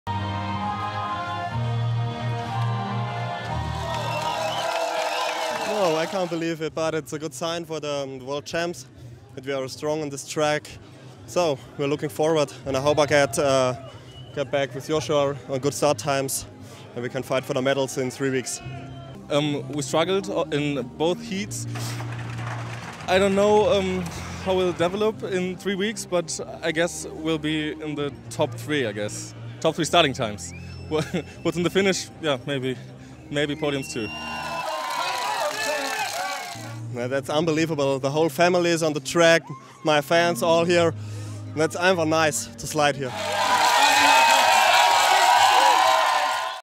Johannes Lochner: "A good sign for the future" | IBSF Official
Bobsleigh pilot Johannes Lochner has achieved victory in the two-man bobsleigh race at the BMW IBSF World Cup in Königssee.